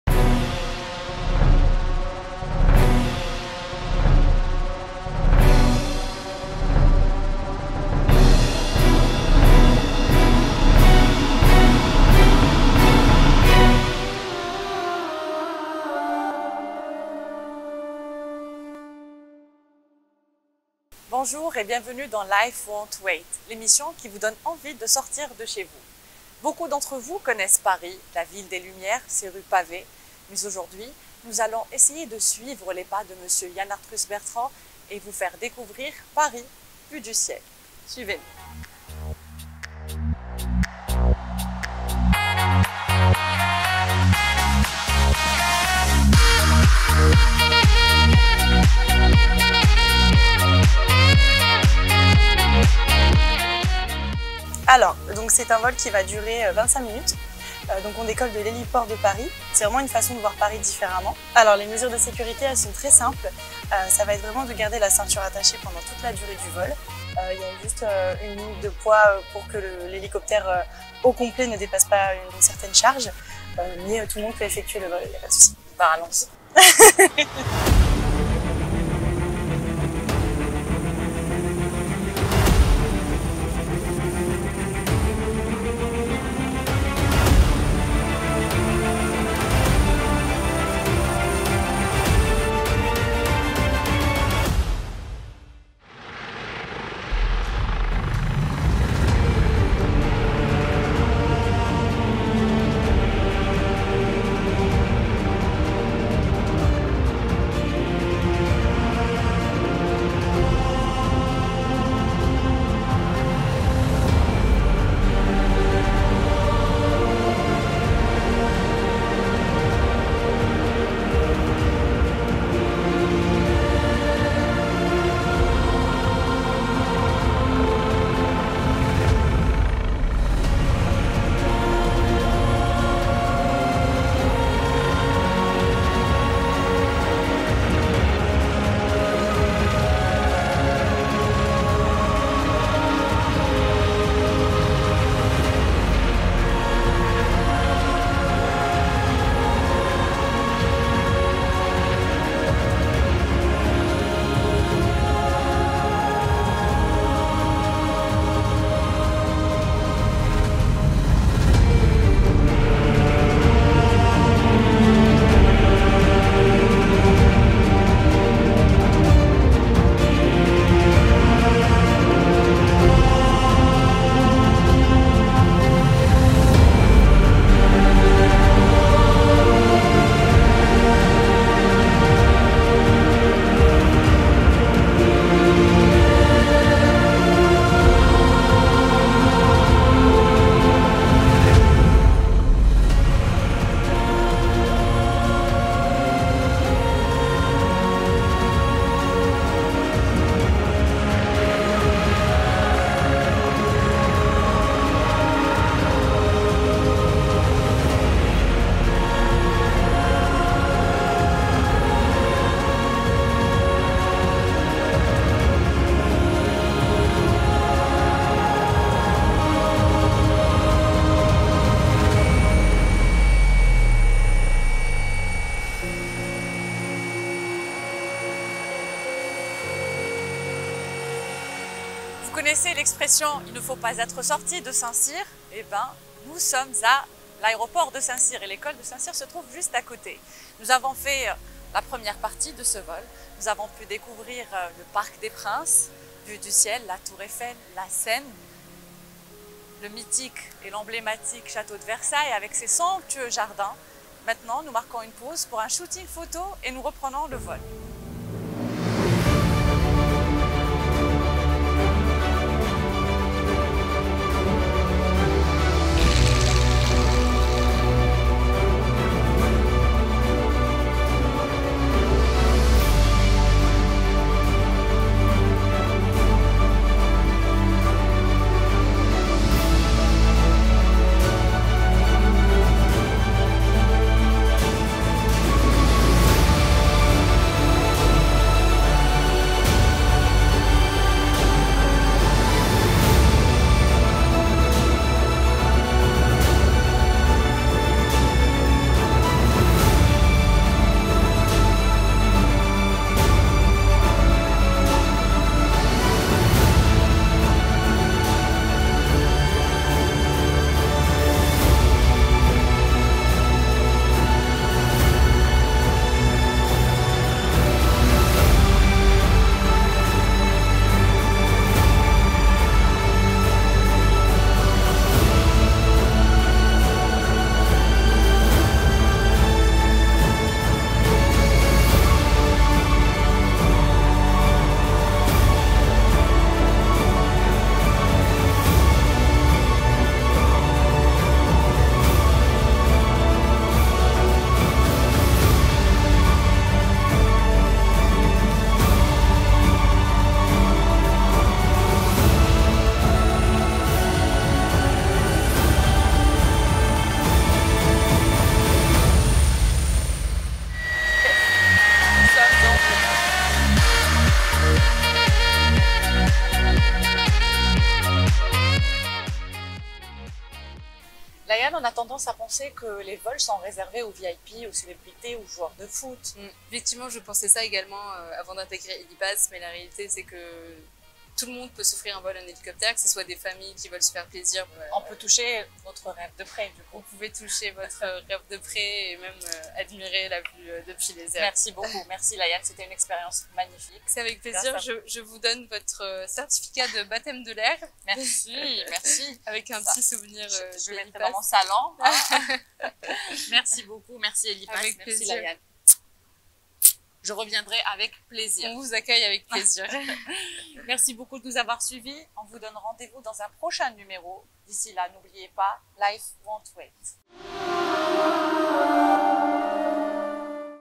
Life Won't Wait - Paris vu du ciel avec Helipass
✨ Découvrez Paris vu du ciel avec Hana Ghezzar Bouakkaz !
Retrouvez Life Won't Wait chaque lundi à 19h sur Facebook, Instagram et Youtube